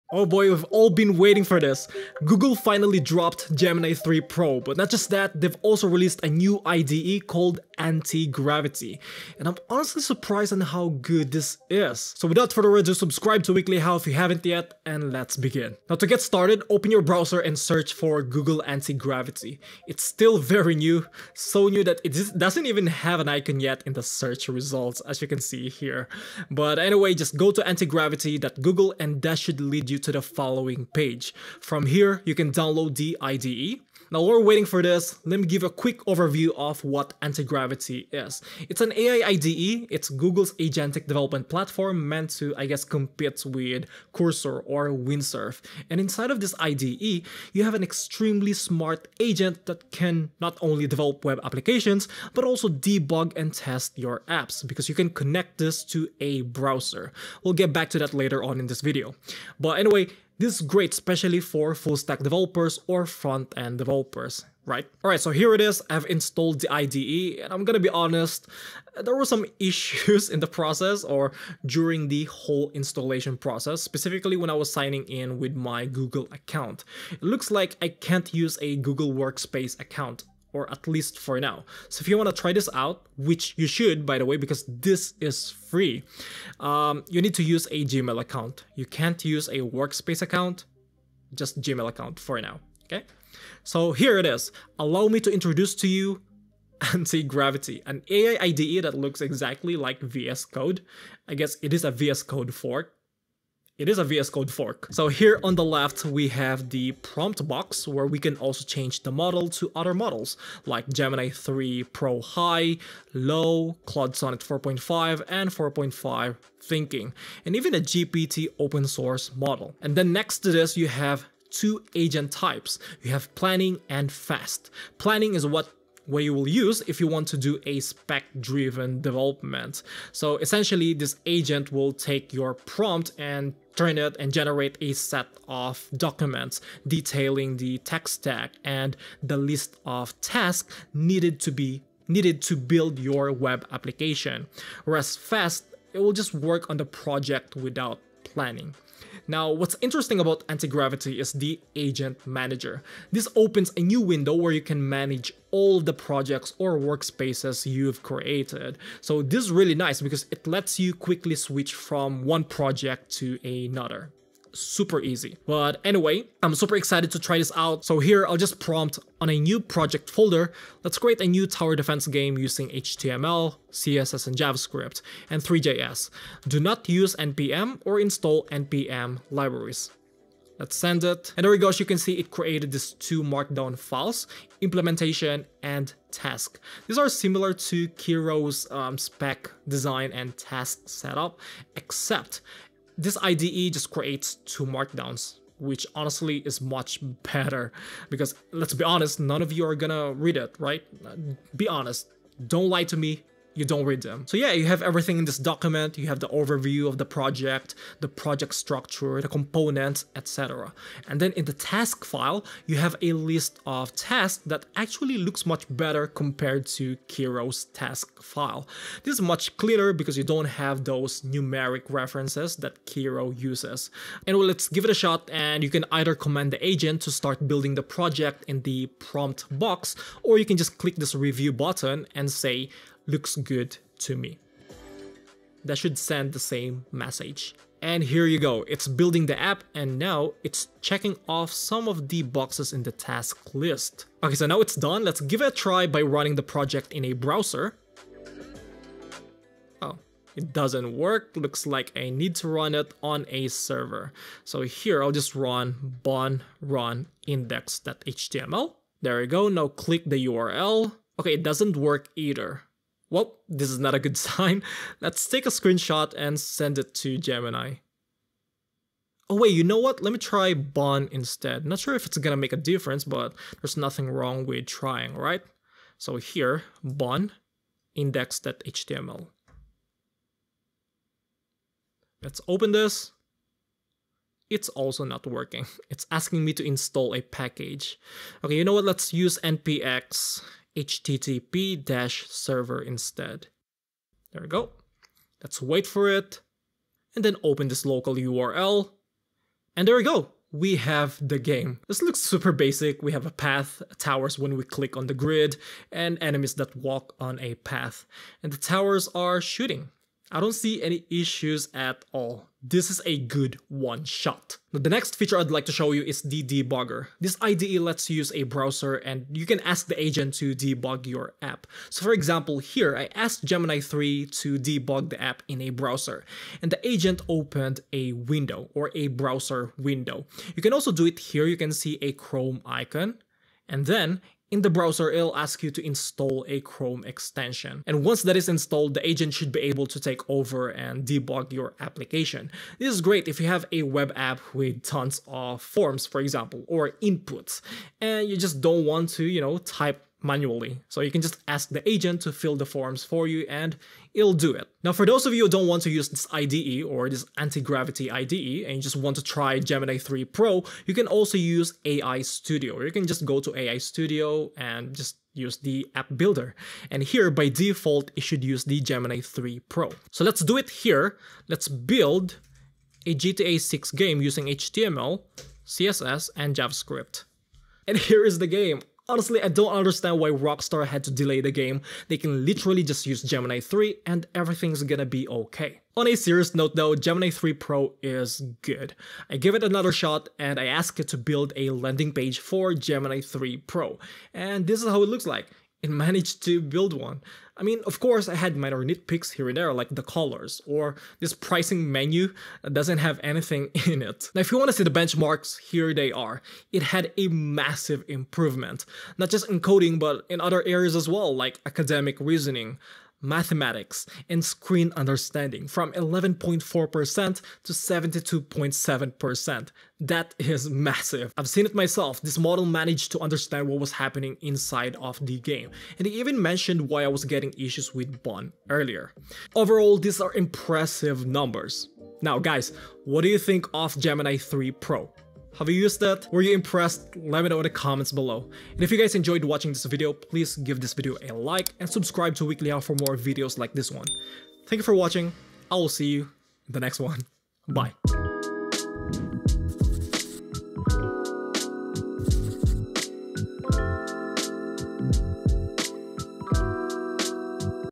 Google’s New Antigravity IDE + Gemini 3 Pro Might Kill Cursor
Google just dropped Gemini 3 Pro AND a brand new AI-powered IDE called Antigravity, and honestly… it’s way better than I expected.

🎥 Grok 4 is out! Can it make Shopify apps?

Timestamps:
0:00 - Google released Gemini 3 Pro & a new IDE?
0:26 - Downloading Google's Antigravity IDE
1:05 - Antigravity First Look
1:45 - Changing Models & Different Agents
2:30 - Agent Manager
2:48 - Building/Planning a project using Antigravity
5:14 - Built-in Browser / Browser Control
5:54 - Building Apps with Gemini 3 Pro (AI Studio)
6:28 - Building a landing page with Gemini 3 Pro
6:44 - Benchmarks
7:09 - Thoughts?